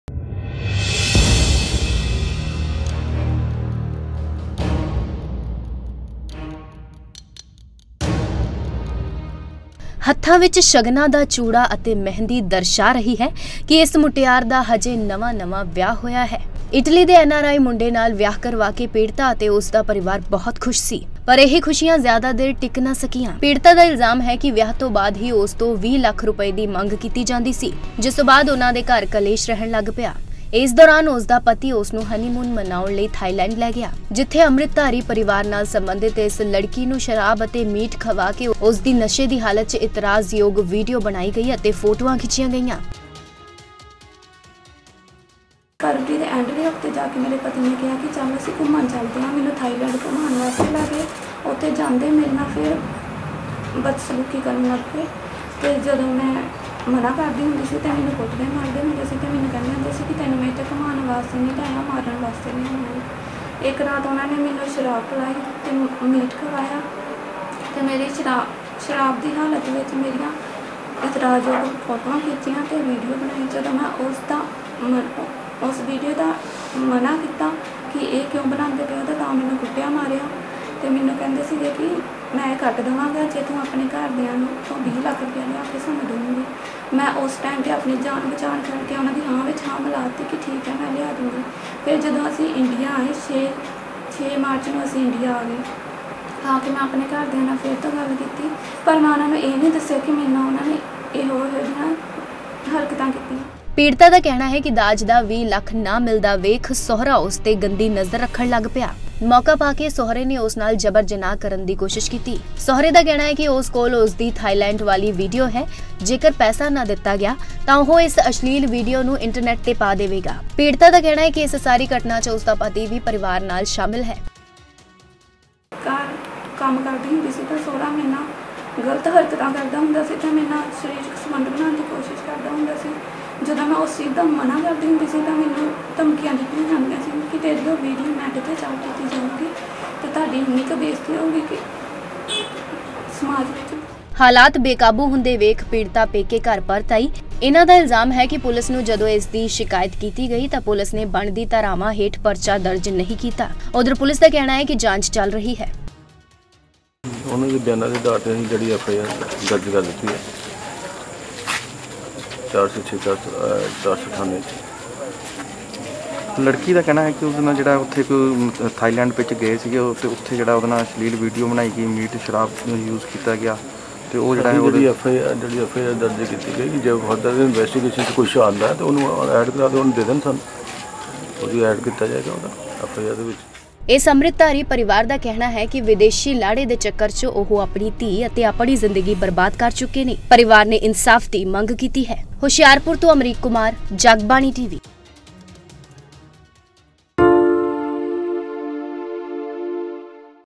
दहेज़ की खातिर N.R.I पति ने पत्नी की बनाई अश्लील Video
ਹੱਥ ਵਿੱਚ ਸ਼ਗਨਾਂ ਦਾ ਚੂੜਾ ਅਤੇ ਮਹਿੰਦੀ ਦਰਸ਼ਾ ਰਹੀ ਹੈ ਕੀ ਇਸ ਮੁਟਿਆਰ ਦਾ ਹਜੇ ਨਵਾਂ ਨਵਾਂ ਵਿਆਹ ਹੋਇਆ ਹੈ.ਇਟਲੀ ਦੇ ਐਨ ਆਰ ਆਈ ਮੁੰਡੇ ਨਾਲ ਵਿਆਹ ਕਰਕੇ ਪੀੜਤਾ ਅਤੇ ਉਸਦਾ ਪਰਿਵਾਰ ਬਹੁਤ ਖੂਸ਼ ਸੀ.ਪਰ ਇਹ ਖੂਸ਼ੀਆ ਜ਼ਿਆਦਾ ਦੇਰ ਟਿੱਕ ਨਾ ਸਕੀਆ.